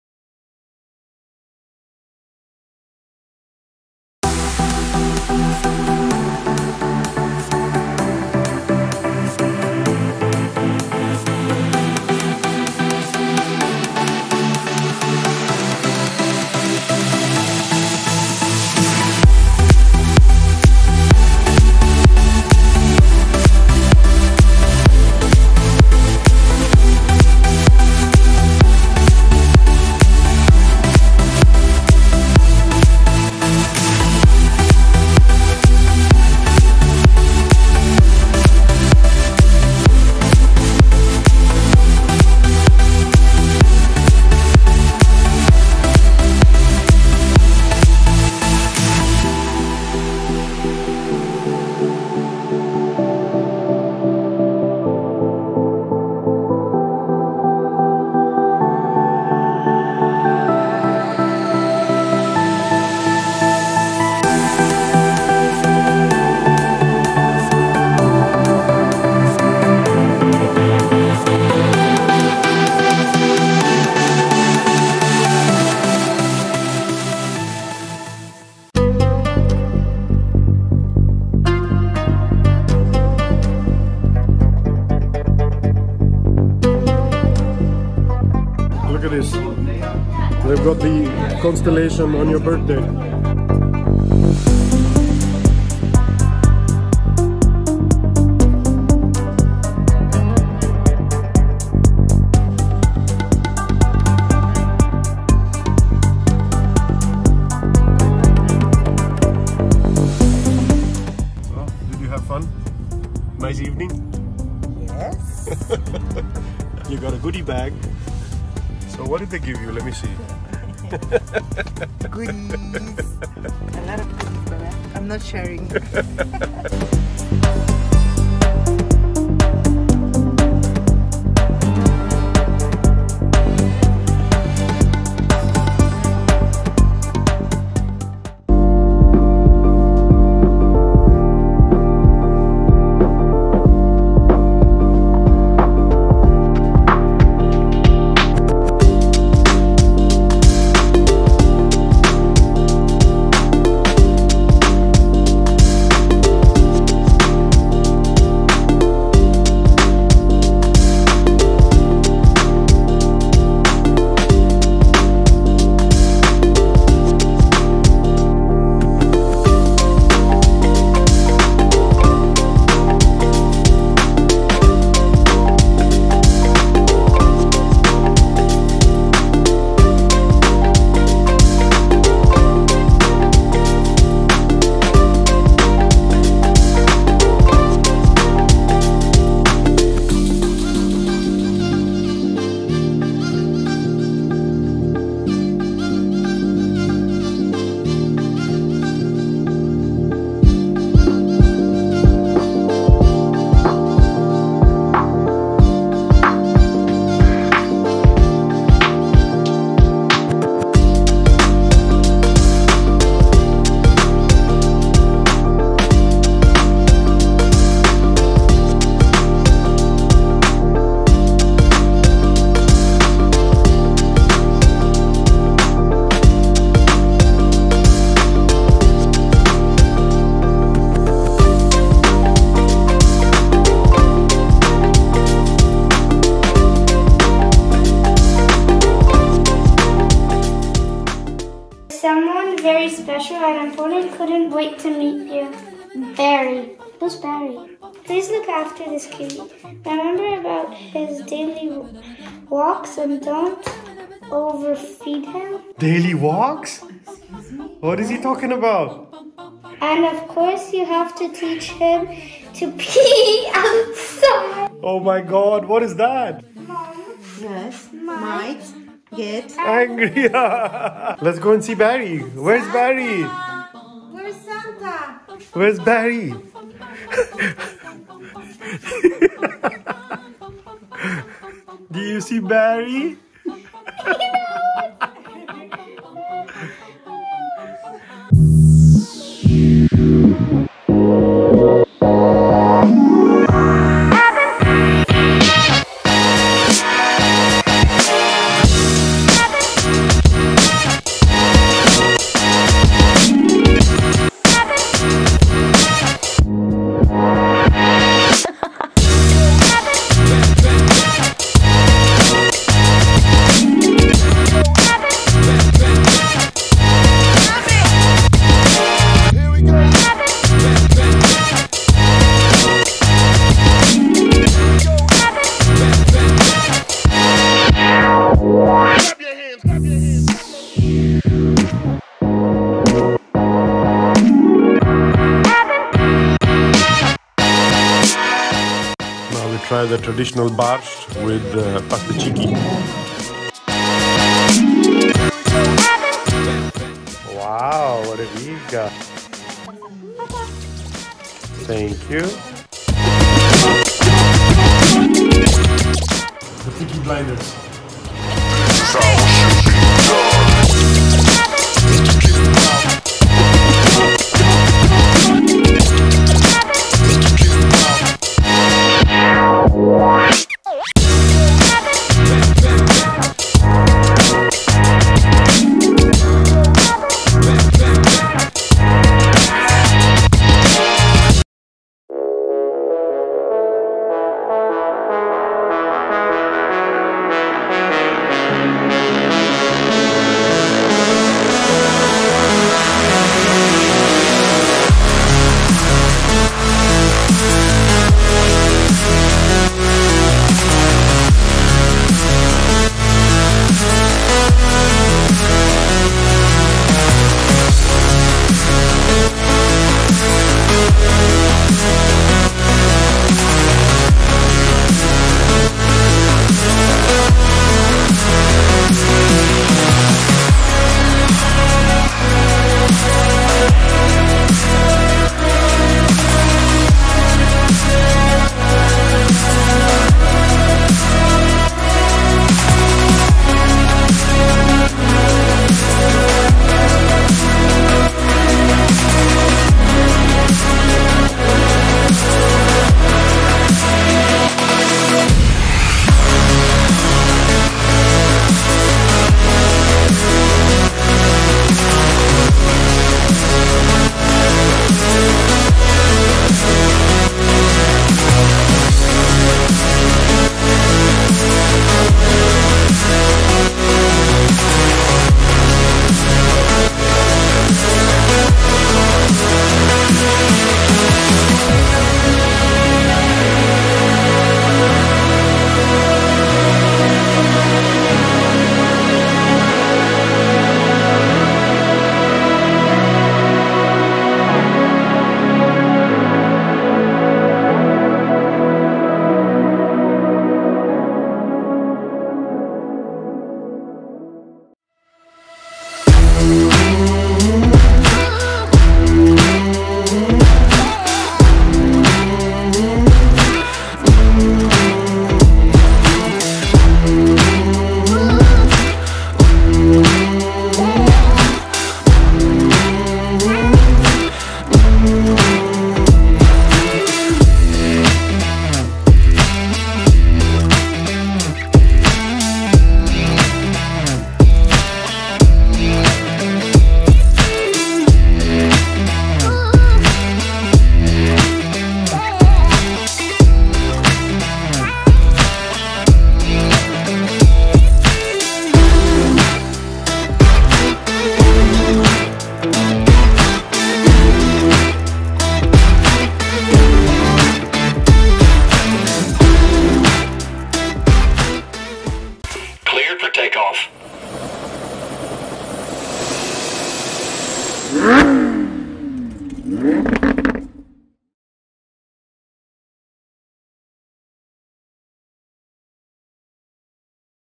#44 Meet Barry 🇵🇱
▶ In this episode, Richie and his family visit the London City Christmas Festival/Experience. Afterwards they head for the Gucci Dinner in Marbella. Furthermore they embark on a journey to Poland to Celebrate Christmas where Richie's daughter gets a whole new friend from Santa!
We finish of this episode with some more private jet flights in the Citation Mustang back and forth to Farnborough in London, UK :-)

00:00 Christmas in London
01:21 Gucci Dinner in Marbella
02:20 Flying to Poland for Christmas
04:01 Meeting Barry on Christmas
06:24 Flying back to the UK
08:17 Flying to Farnborough